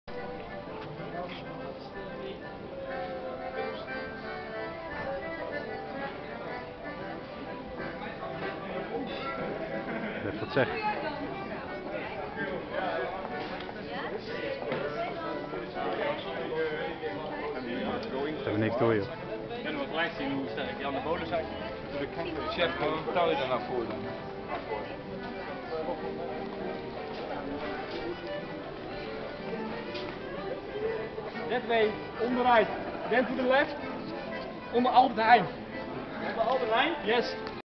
Foute Vrienden spel in Apeldoorn op 26-08-2017 (nr. 51)
Op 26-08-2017 was het Foute Vrienden  spel in Apeldoorn als Vrijgezellenfeest of Vrijgezellenfeesten